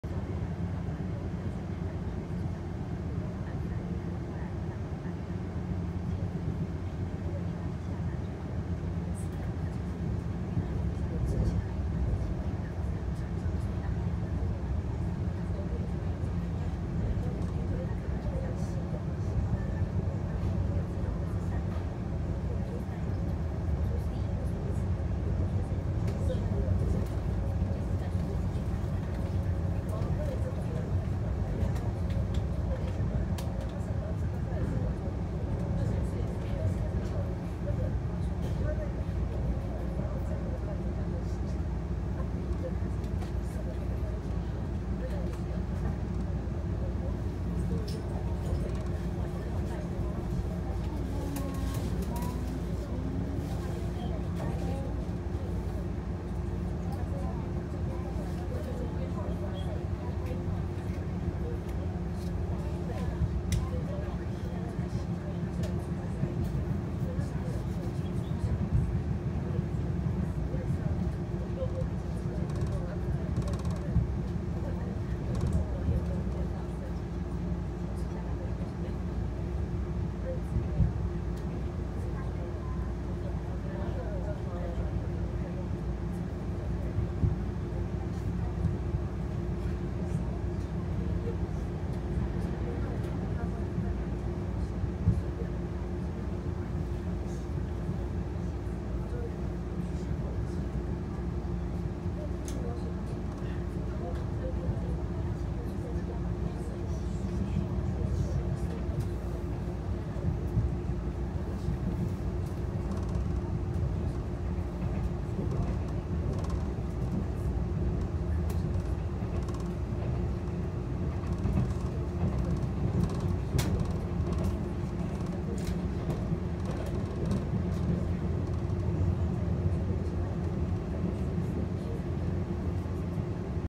415次普悠瑪自強號枋寮站1B月台通過(交會3062次區間快車(EMU800替駛))
(台東、花蓮、樹林端)TED2013－TED2014(新左營端)。